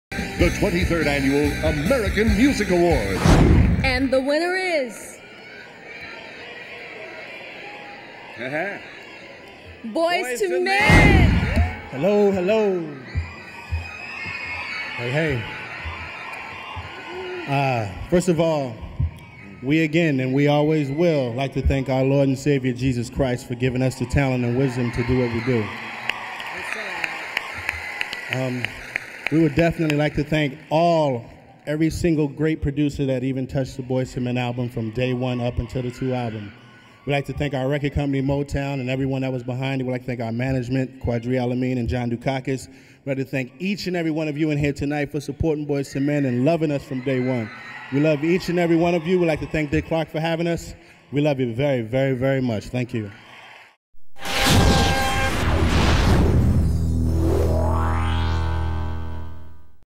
Boyz II Men win R&B Favorite Album - AMA 1996
Boyz II Men accept the R&B Favorite Album at the American Music Awards in 1996.